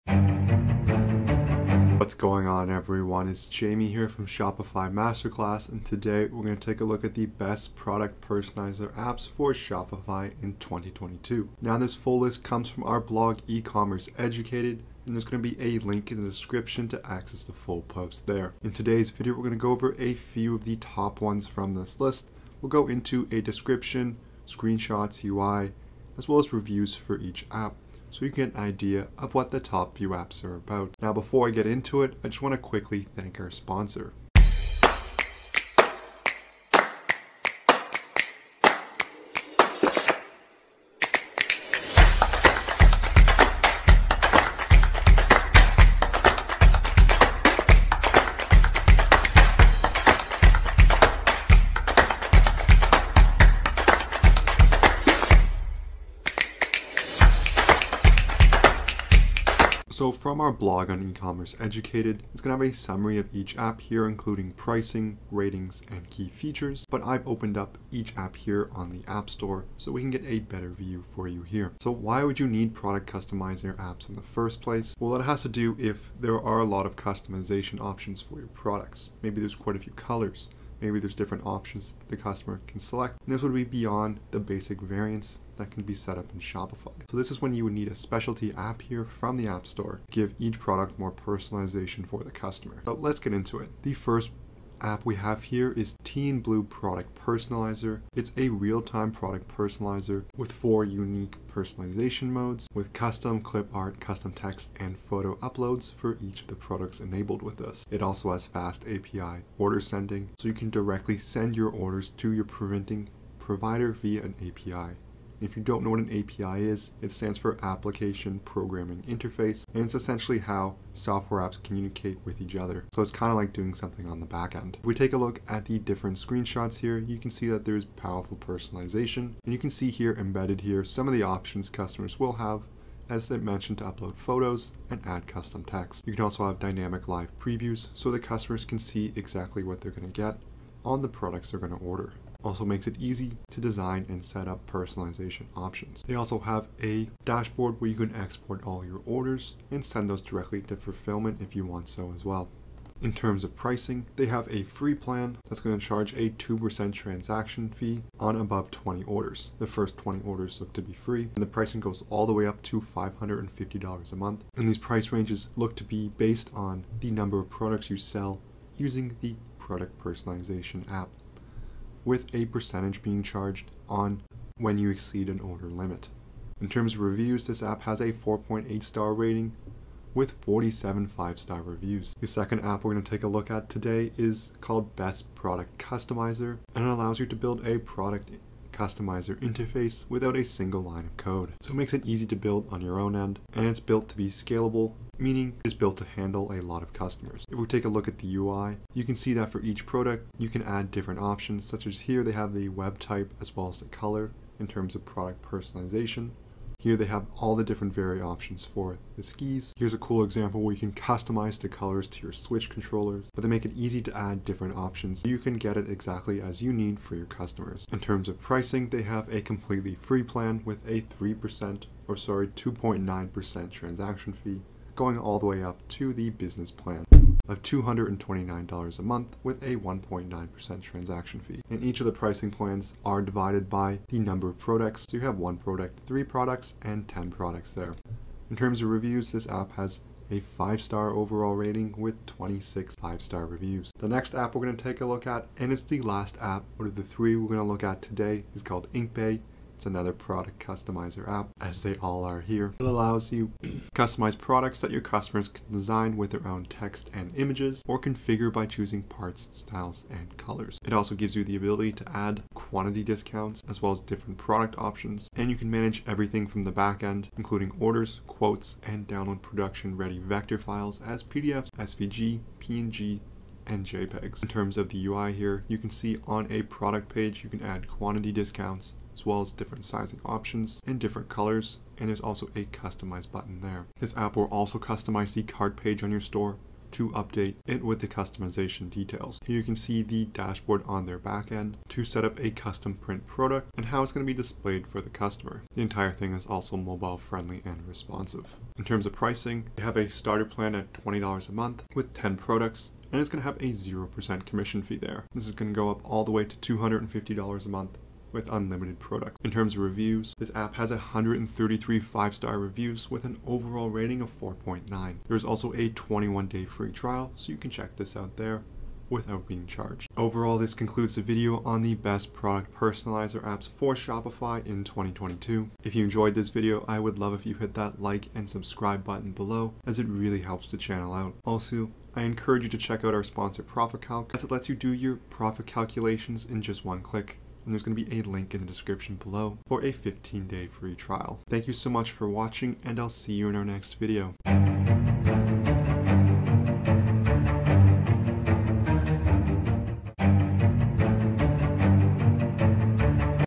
Shopify Product Personalizer Apps: Product Personalizer Shopify Tutorial
In this video, we'll show you the best product personalizer apps available on Shopify.

Product personalization can give your customers amazing options to make their products their own. This increases their incentive to buy because the product will mean more to them. However, Shopify's default product setup isn't built for product personalization. That's why we've collected the best free and paid product personalizer apps for Shopify.

📈 Ready to Calculate Your Shopify Profit In Just One-Click? (Free 15-day Trial for Accounting App)